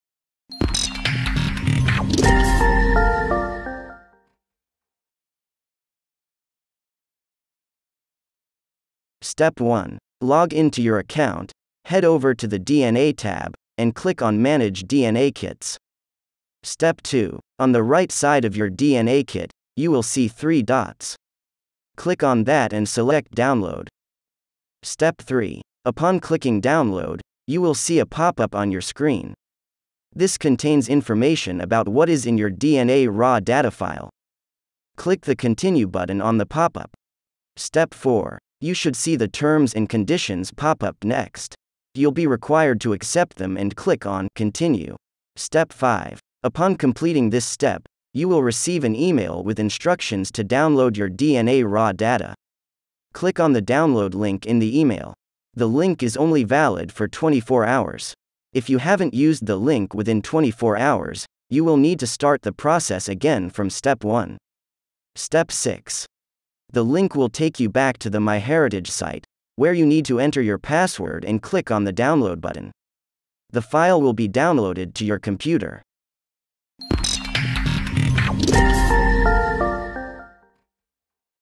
How To Download Your MyHeritage DNA Raw Data File?
Watch this video to find out how you can download your MyHeritage DNA raw data file in six simple steps!

Learn how you can use this data with Xcode Life for 13 additional health reports:

Introduction (00:00)
Kit Selection for DNA Raw Data Download (00:17)
Email With Link to Download Your Data (00:46)
Final Step of the Download (01:06)